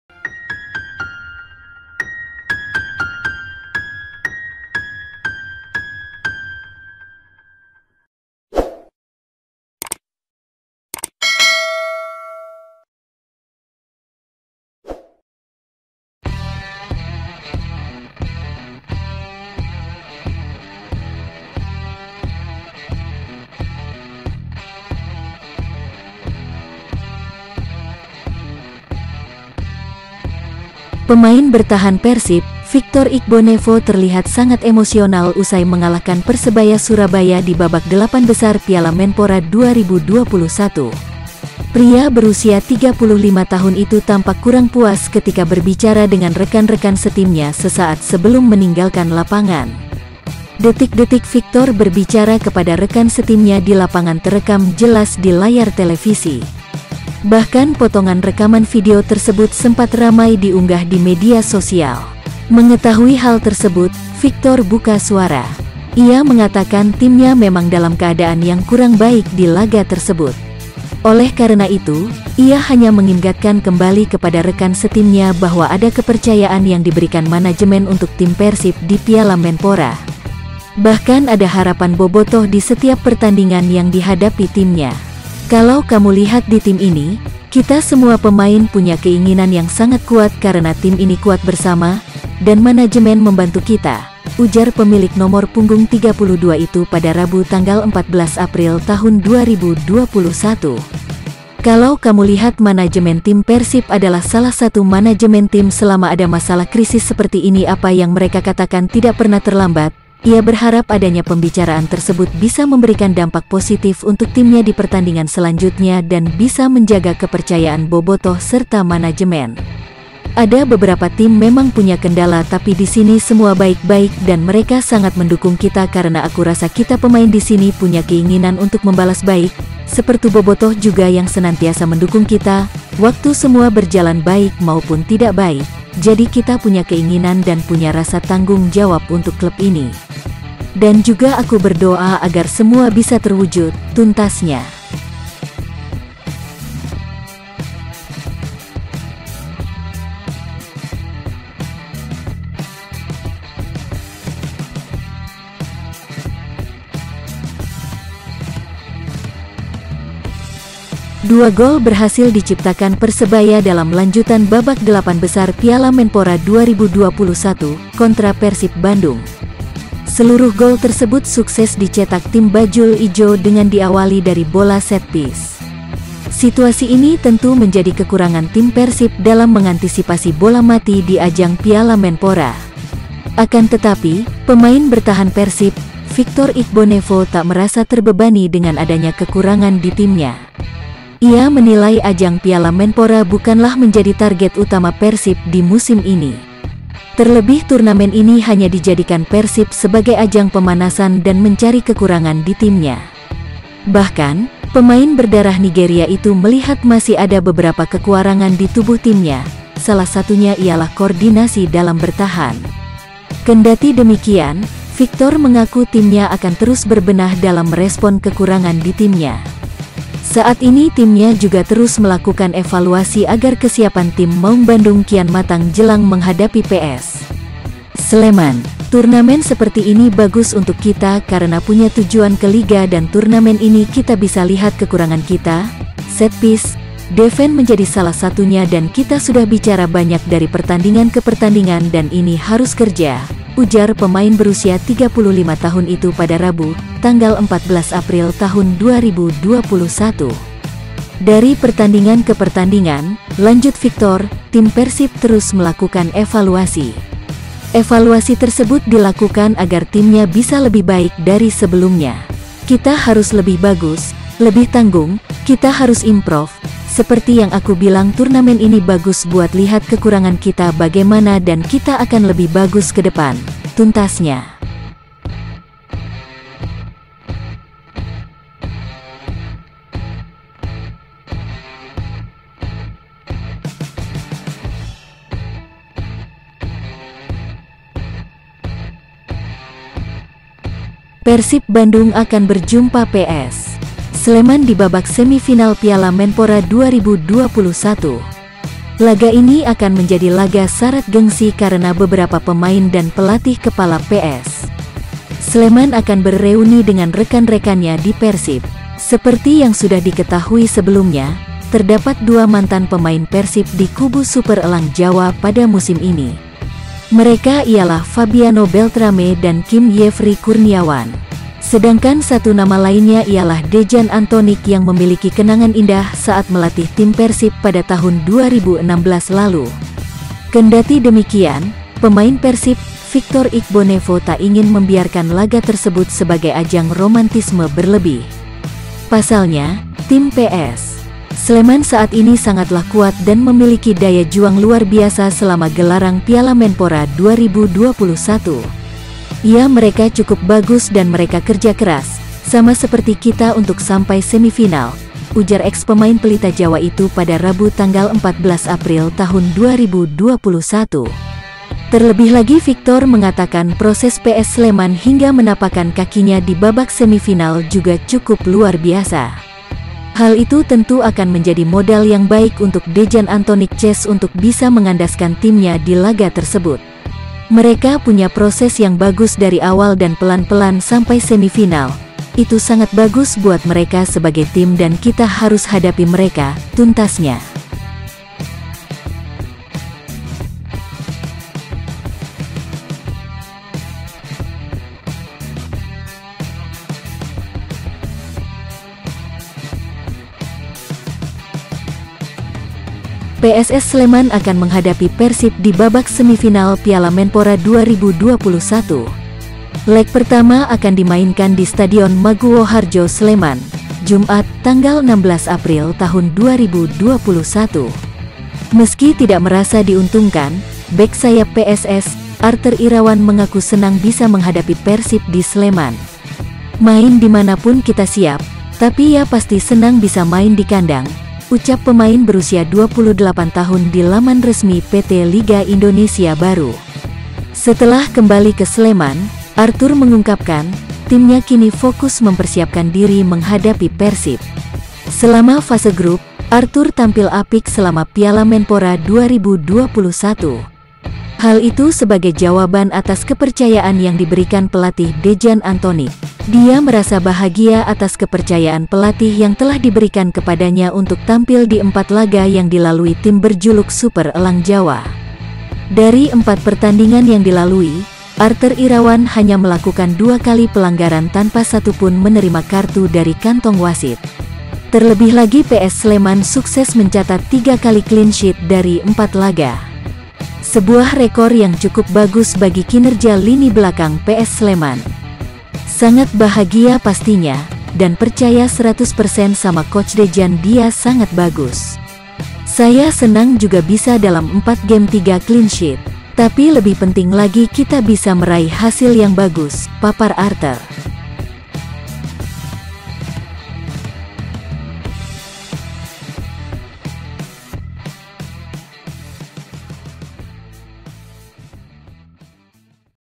BERITA PERSIB TERBARU HARI INI 15 APRIL 2021 PART 2
1. Tampak Emosional Usai Kandaskan Persebaya, Begini Kata Victor Igbonefo
2. Persib Masih Banyak Kekurangan, Victor: Turnamen Ini Bagus Buat Lihat Kekurangan Kita
3. PS Sleman Punya Proses Yang Bagus Ke Semi Final
4. Piala Menpora: Pemain PSS Ini Senang Bisa Hadapi Persib di Sleman